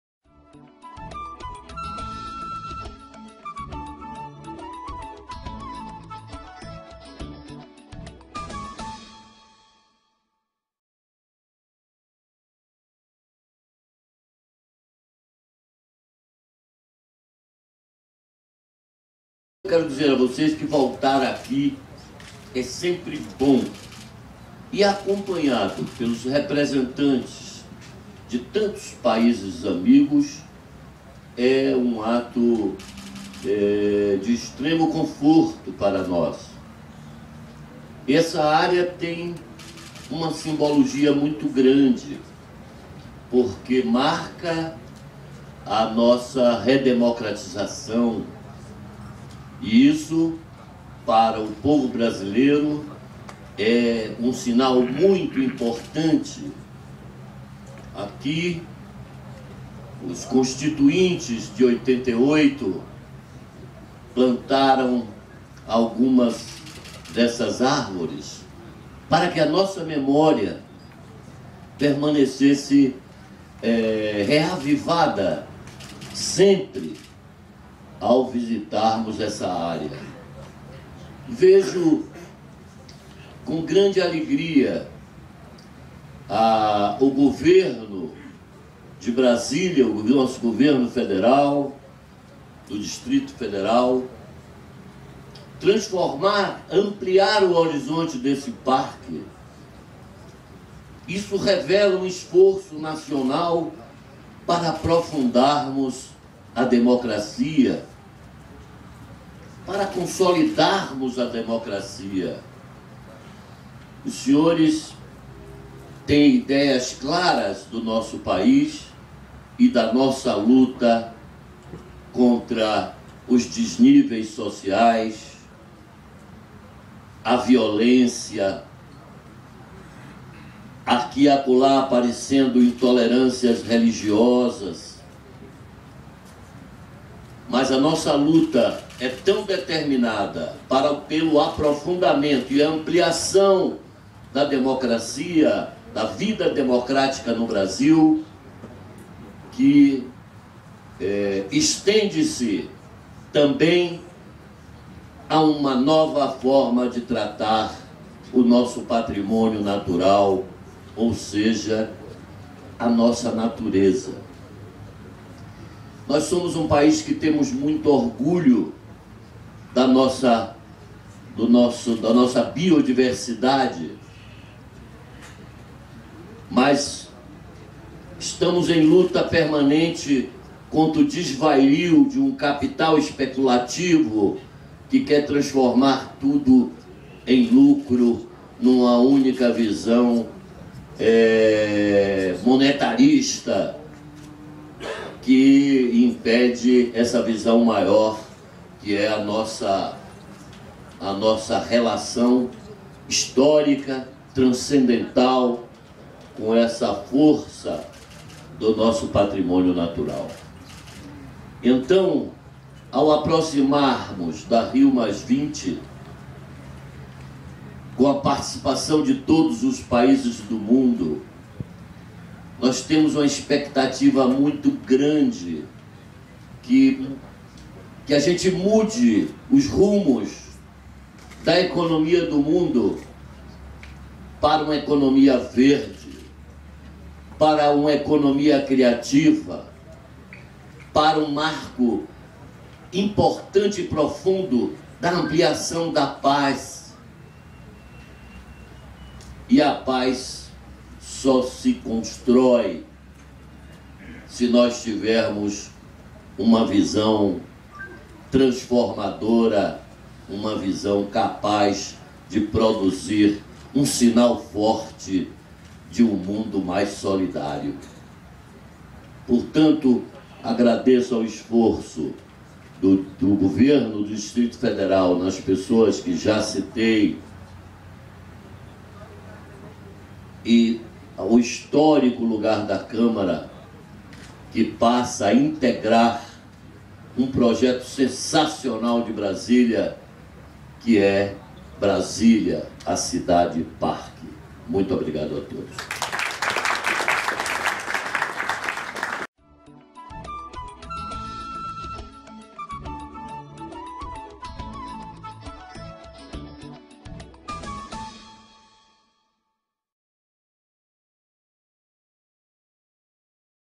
Penna no plantio do "Arboreto das Nações da Rio+20" - 31/05/2012
O deputado Penna (PV-SP) participou na manhã desta quinta-feira do plantio do "Arboreto das Nações da Rio+20", no Parque Bosque dos Constituintes, localizado próximo à Praça dos Três Poderes, em Brasília. A solenidade contou com a presença de representantes dos 120 países que participarão da Conferência da ONU, e cada um plantou uma muda de árvores nativas do cerrado. "Voltar aqui é sempre bom, e acompanhado pelos representantes de tantos países amigos é um ato de extremo conforto para nós", celebrou Penna, que discursou representando a Comissão de Meio Ambiente e Desenvolvimento Sustentável da Câmara, da qual é vice-presidente.

O plantio desta manhã remontou ao ato da criação do Bosque dos Constituintes. As primeiras mudas da área foram plantadas pelos deputados que integraram a Assembleia Nacional Constituinte, daí o nome do parque. "Esta área tem uma simbologia muito grande, porque marca a nossa redemocratização, e isto para o povo brasileiro é um sinal muito importante. Aqui, os constituintes de 88 plantaram algumas dessas árvores para que a nossa memória permanecesse reavivada sempre ao visitarmos essa área", lembrou o deputado.

Com a presença do presidente da Câmara, Marco Maia, do vice-governador do DF, Tadeu Filippelli, e do secretário de Meio Ambiente, Eduardo Brandão, a solenidade também marcou a assinatura de documentos para a ampliação do Bosque dos Constituintes e para a adesão definitiva da Câmara dos Deputados às ações do programa "Brasília, Cidade Parque". "Vejo com grande alegria o governo do DF ampliar o horizonte deste parque. Isto revela um esforço nacional para aprofundarmos a democracia, para consolidarmos a democracia", elogiou Penna.

"Os senhores têm ideias claras do nosso país e da nossa luta contra os desníveis sociais, a violência, as intolerâncias religiosas. Mas a nossa luta é tão determinada pelo aprofundamento e ampliação da democracia, da vida democrática no Brasil, que se estende também a uma nova forma de tratar o nosso patrimônio natural, a nossa natureza", argumentou o deputado. "Temos muito orgulho da nossa biodiversidade, mas estamos em luta permanente contra o desvario de um capital especulativo que quer transformar tudo em lucro, numa única visão monetarista", criticou.

"Com a proximidade da Rio+20, nós temos uma expectativa muito grande que a gente mude os rumos da economia do mundo para uma economia verde, criativa, para um marco importante e profundo da ampliação da paz", afirmou. "A paz só se constrói se nós tivermos uma visão transformadora, capaz de produzir um sinal forte de um mundo mais solidário", concluiu o deputado.